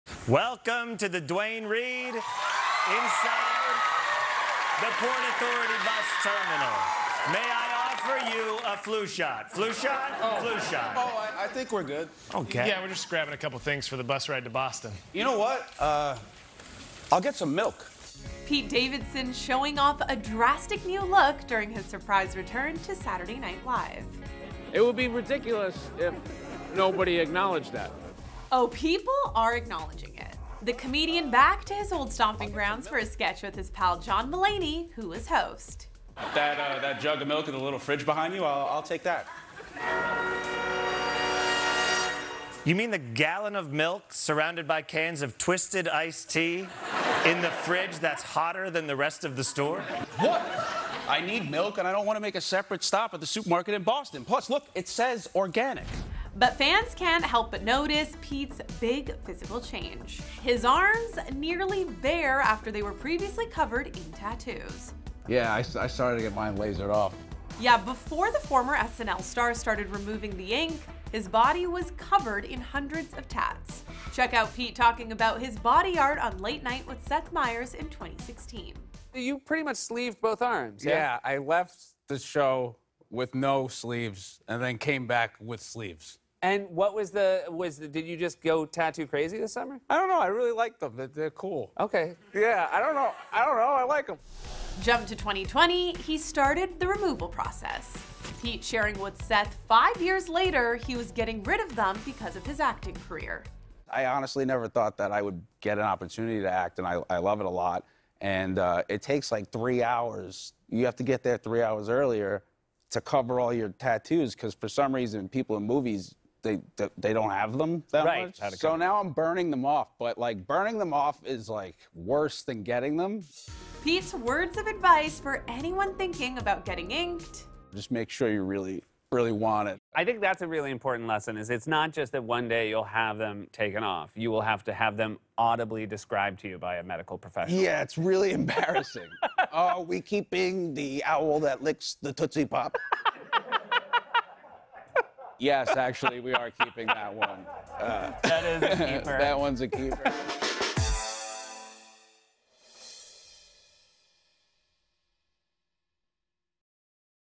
Pete Davidson Rocks DRASTIC New Look During UNEXPECTED SNL Return
Pete Davidson showed off a drastic new look while making a surprise return to 'Saturday Night Live' with host John Mulaney. The comedian showed off the results of his extensive tattoo removal, revealing his arms are now nearly-bare after being completely covered in ink.